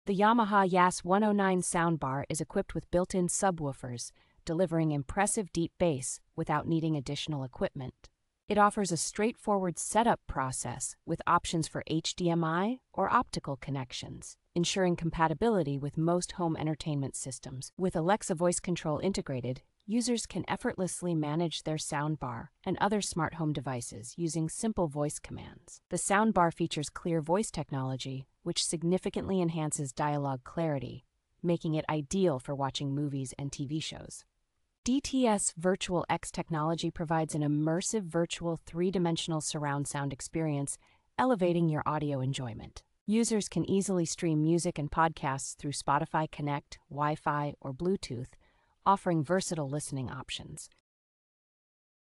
REVIEW (2025): Yamaha YAS-109 Sound Bar. ESSENTIAL details.
The Yamaha YAS-109 Sound Bar is equipped with built-in subwoofers, delivering impressive deep bass without needing additional equipment.

It offers a straightforward setup process with options for HDMI or Optical connections, ensuring compatibility with most home entertainment systems.

With Alexa voice control integrated, users can effortlessly manage their sound bar and other smart home devices using simple voice commands.

The sound bar features clear voice technology, which significantly enhances dialogue clarity, making it ideal for watching movies and TV shows.

DTS Virtual: X technology provides an immersive virtual three-dimensional surround sound experience, elevating your audio enjoyment.

Users can easily stream music and podcasts through Spotify Connect, Wi-Fi, or Bluetooth, offering versatile listening options.